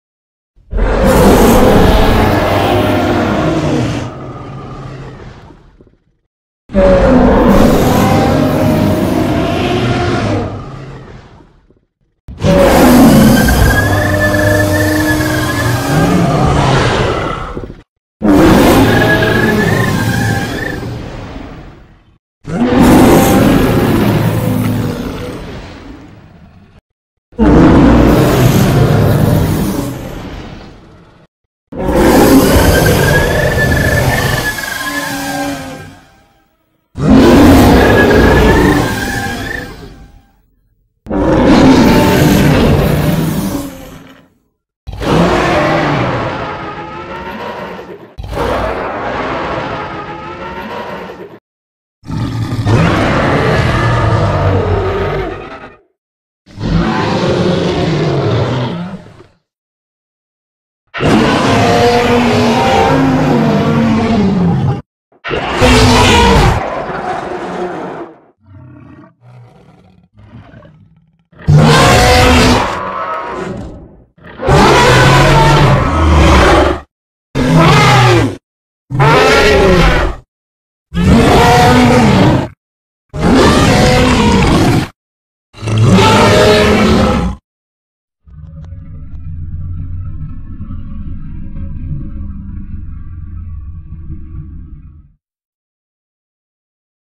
Titanus Kurumu Kurono Sounds
Titanus Kurumu Kurono this Vampire Converted a Dragon titan Kaiju

Trivia:
This Kaiju Like Similar to Rukodiora from Monster Hunter

Dragon Wars: Aftershock: Chapter 1 (Comics)